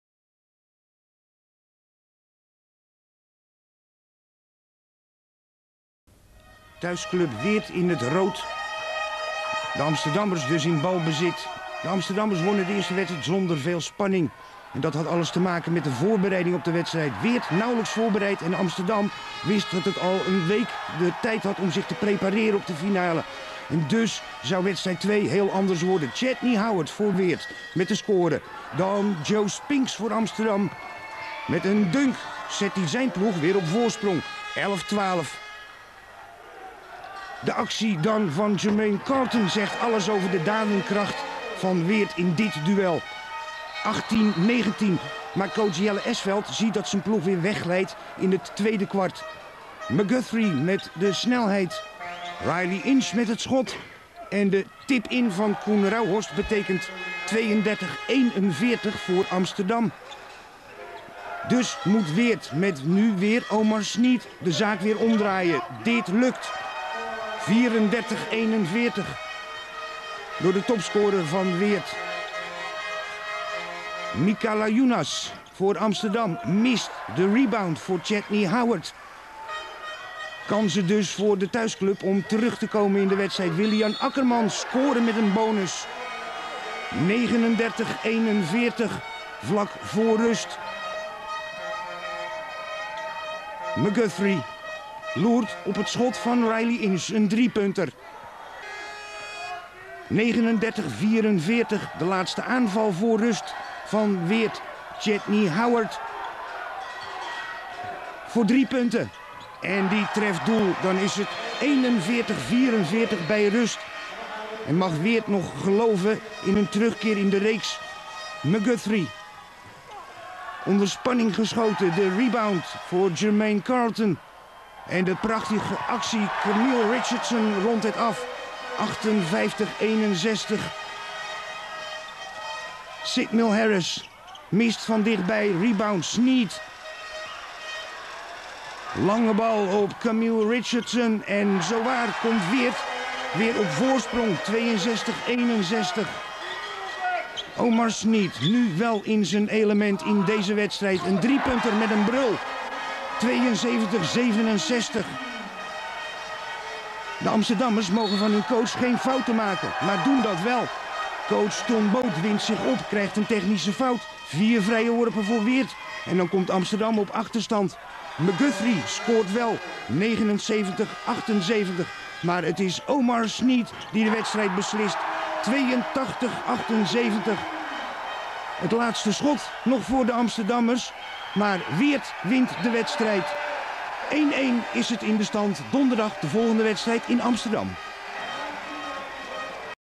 klassieker 12 BSW-Amsterdam 82-78
Verslag van de tweede wedstrijd in de finale van de play-offs tussen BSW en Amsterdam uit het seizoen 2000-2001. BSW won deze wedstrijd met 82-78 en bracht daarmee de stand in de serie op 1-1. Omar Sneed scoorde 30 punten, Chatney Howard 22.